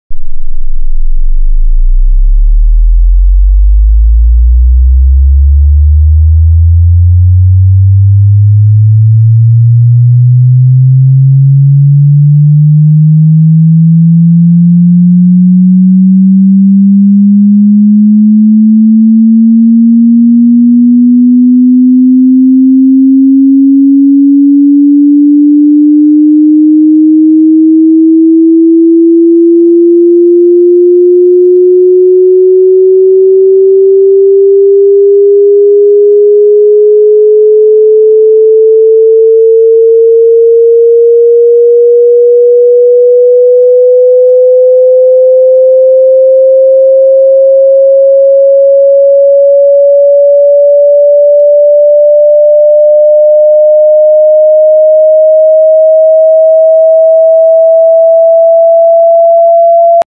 10 - 680 Hz Sweep Tone Audio Sweep Ton wechselnde Frequenz
10 - 680 Hz Sweep Sinus Testton
0 DB Stereo
60 Sekunden
10 - 680  Hz sweep sine test tone
0 DB Stereo
60 sec.